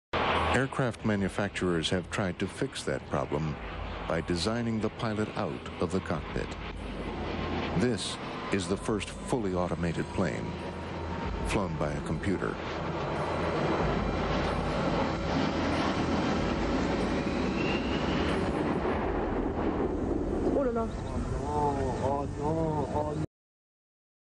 Autopilot Crash
It appears computers are just as good as real pilots when it comes to crashing the airplane.